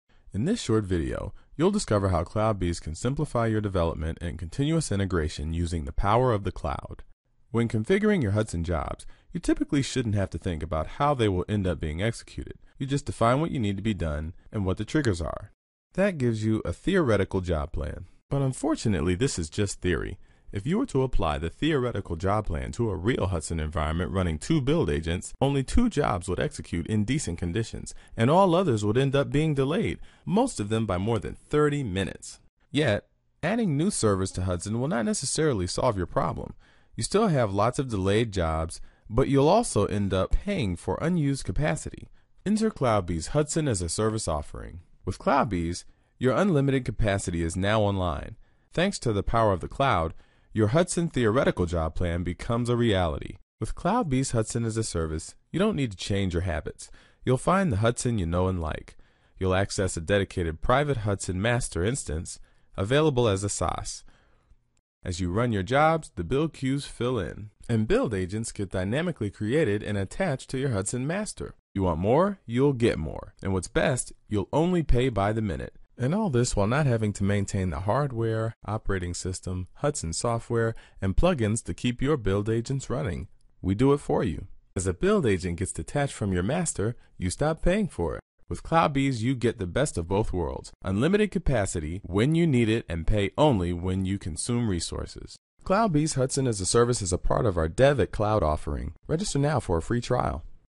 Quick overview of CloudBees' Hudson as a Service (HaaS)
This short video will show you the advantages of CloudBees' Hudson as a Service (HaaS), part of the DEV@cloud offering.

With CloudBees HaaS, you get unlimited build agents, ready in a snap, and you only pay usage by the minute.